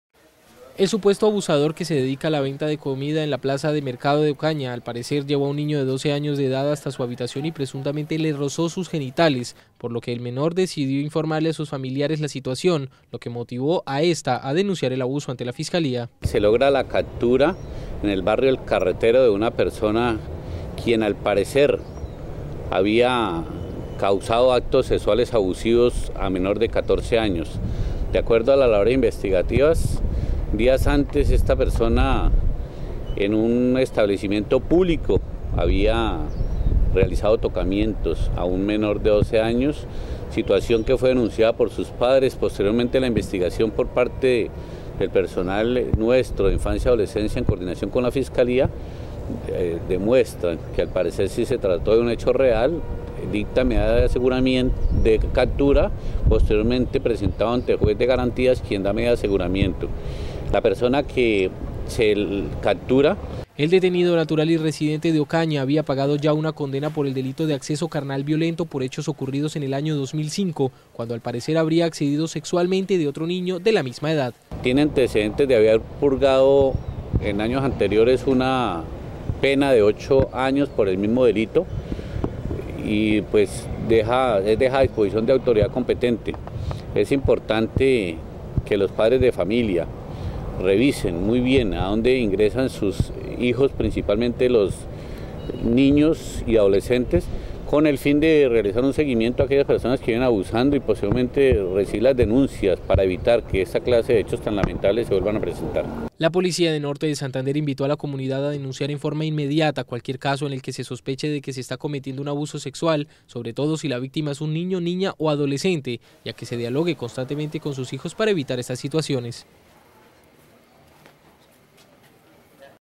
ATN NOTICIAS 12-11 Presunto violador de menores reincidió luego de pagar ocho años de cárcel.
Presunto violador de menores reincidió luego de pagar ocho años de cárcel.